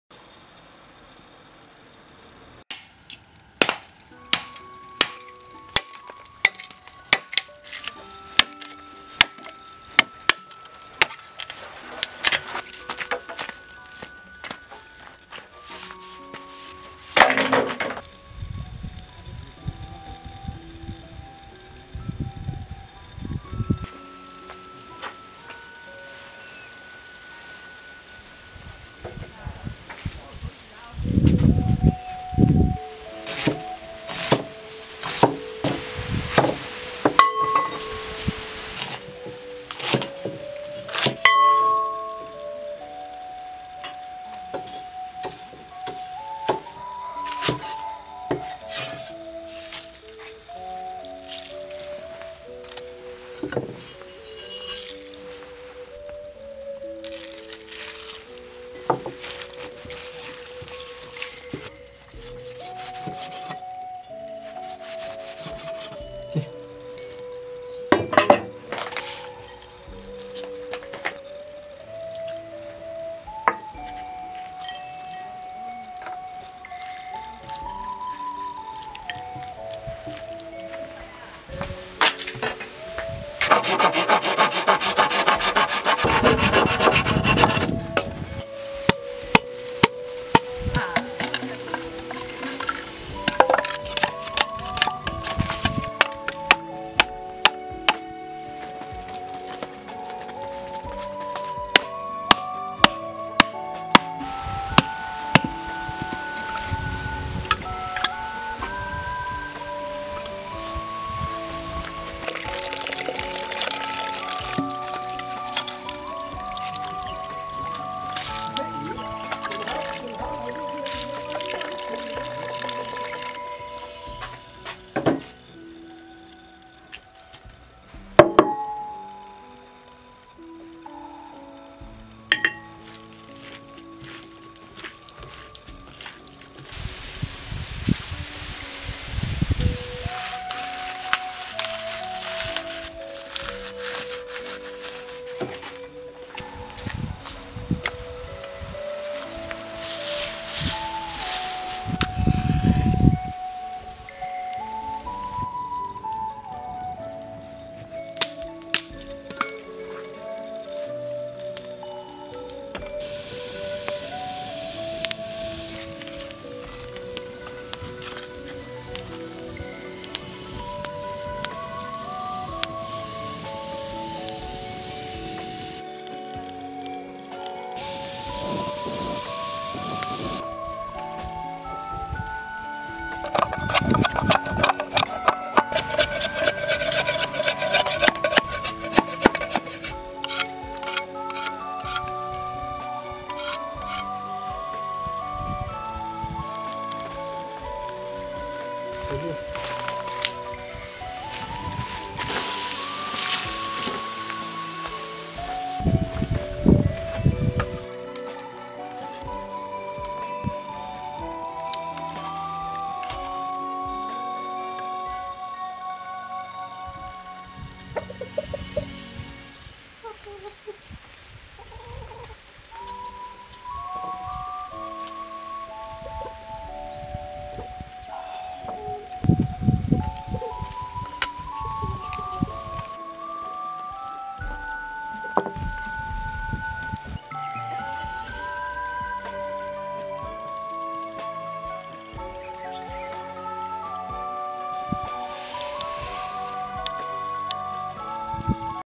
#24 不用煎，不用烤，也能做出香噴噴的牛肉，竹筒牛肉自帶清香
You can make fragrant beef without frying or roasting. Bamboo beef has its own fragrance.